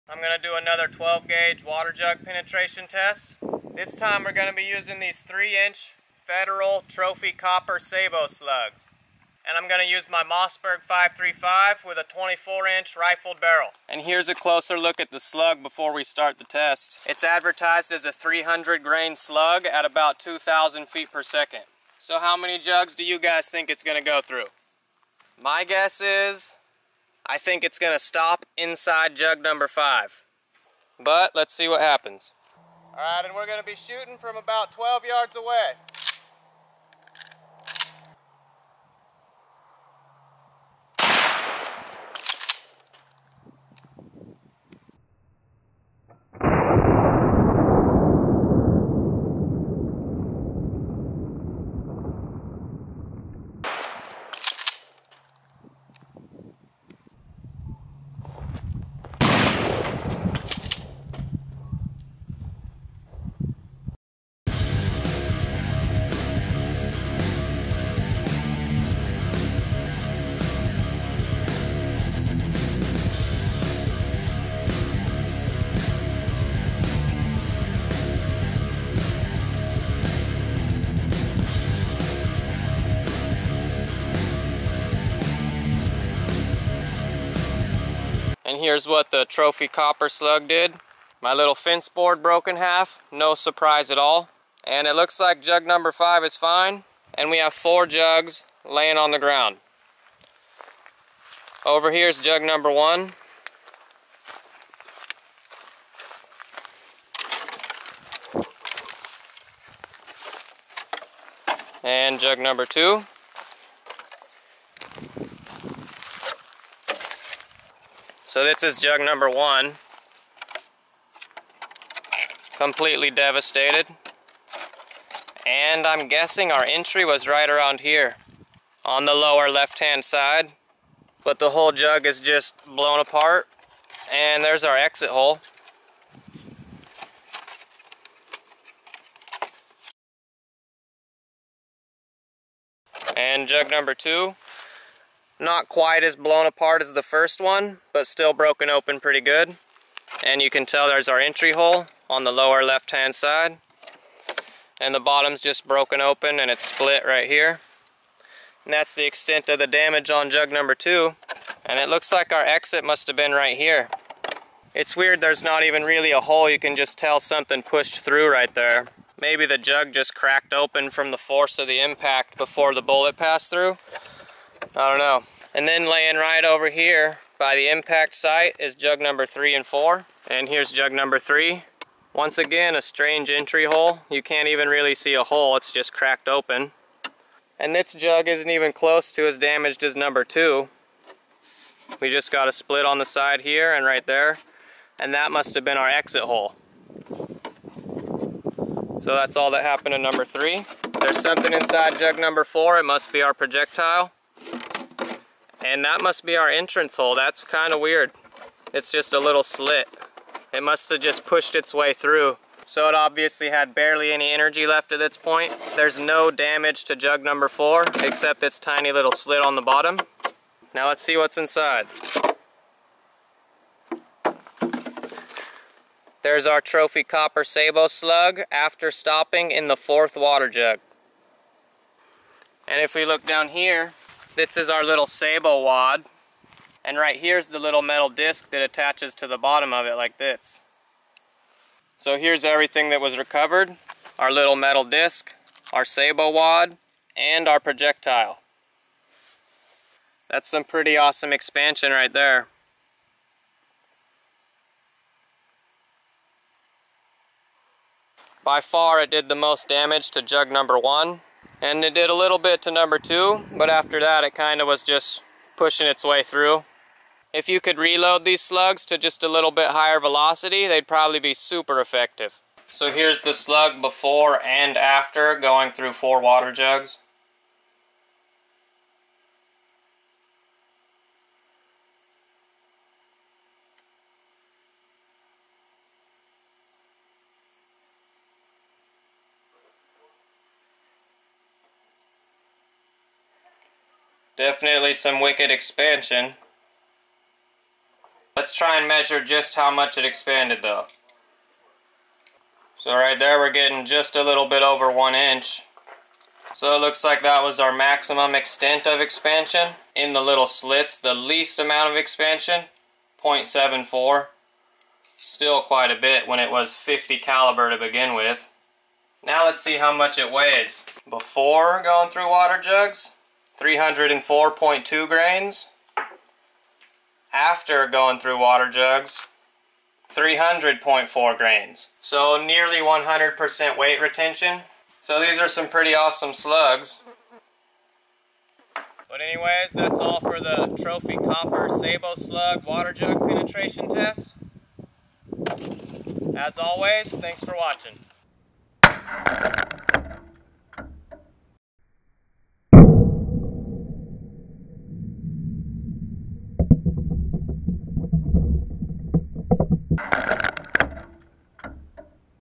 12 Gauge Trophy Copper Sabot Slug Water Jug Penetration Test
Federal 3 inch Trophy Copper Sabot Slug damage and penetration test on water jugs.